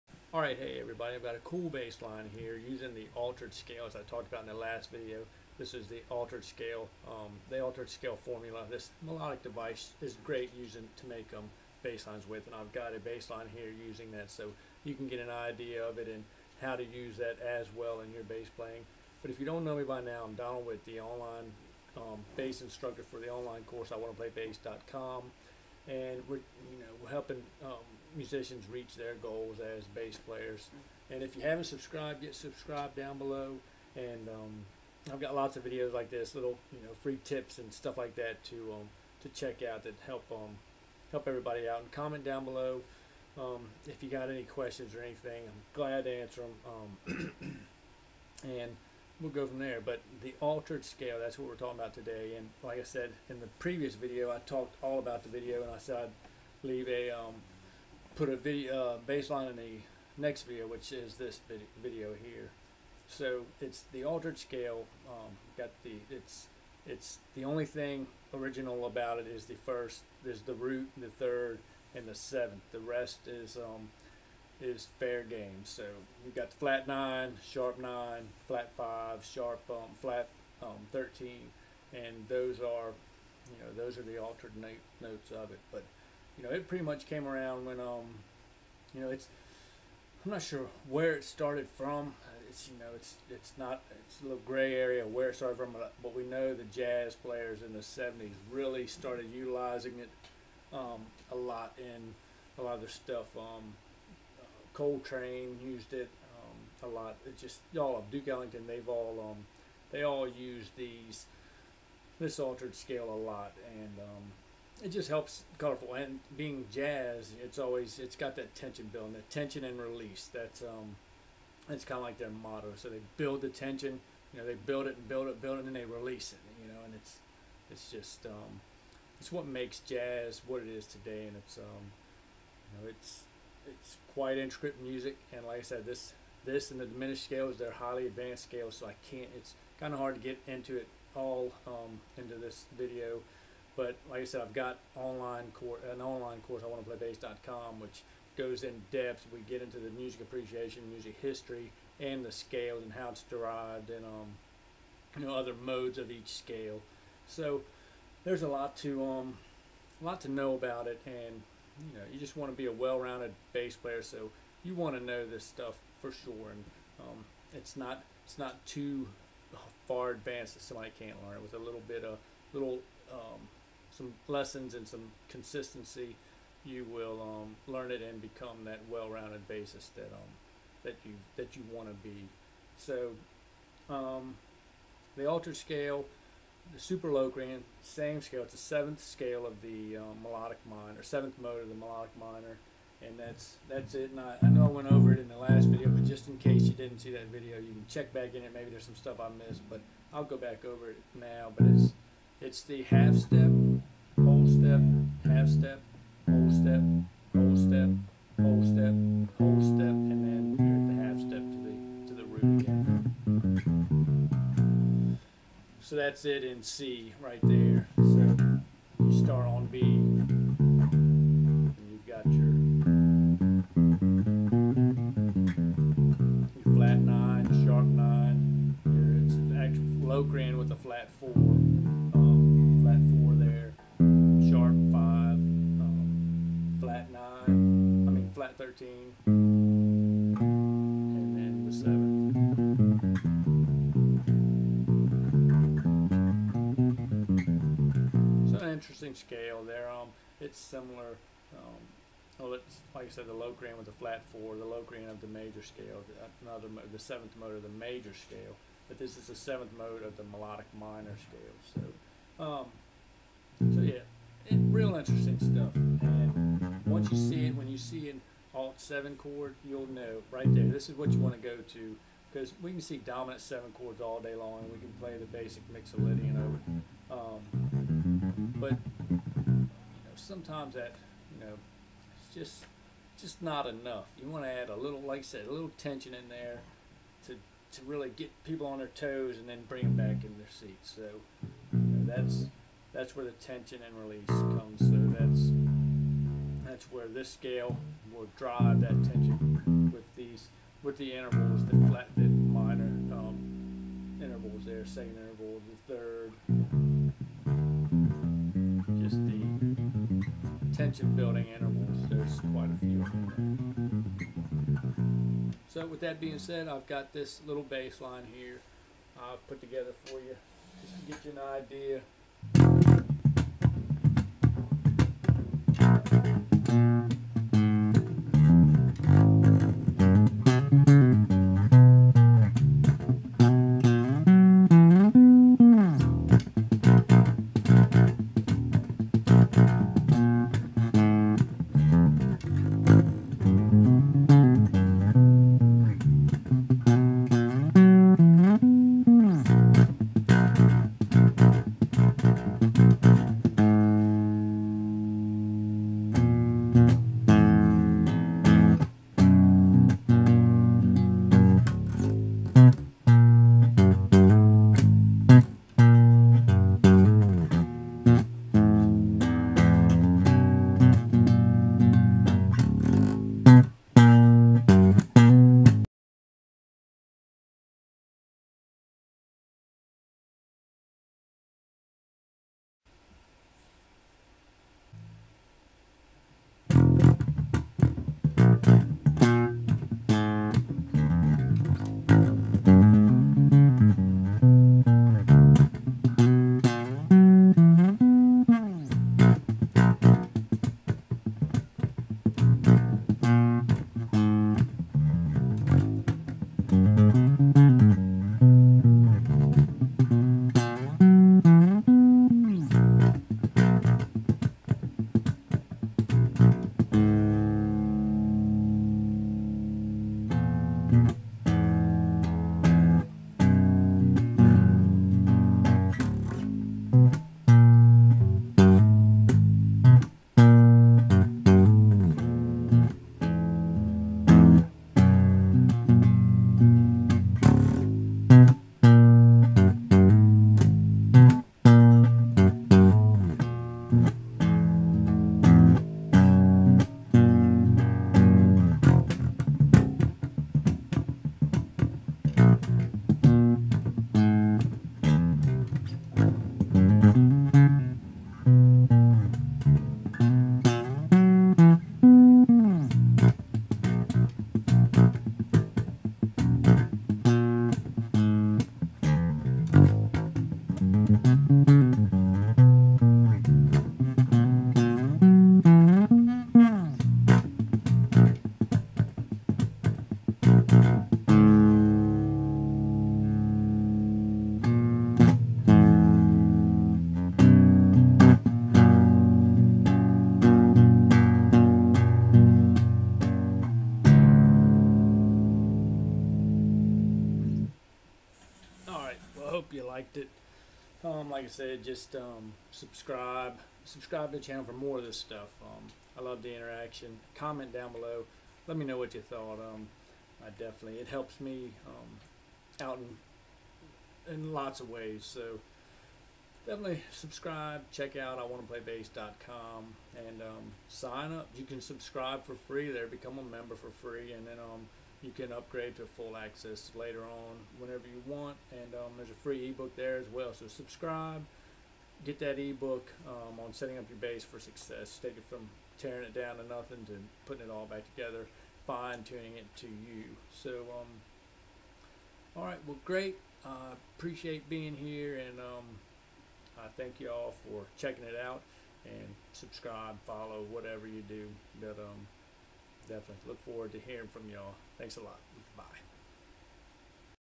The altered scale formula
The altered scale formula for cool bass lines. The altered scale is a melodic device for bass lines, bass solo's and especially for walking bass lines. Used by jazz players for the tension and release effect. Bass lines you should know that use the altered scale effectively over the alt 7 chords and dom 7 chords through the 16251 and 251 chord progressions. Learn these beginning bass lines at I wanna play bass your how to play bass online course. The altered scale also called super locrian the seventh mod of the melodic minor or the locrian flat four the seventh mode of the major scale except it has a flat four.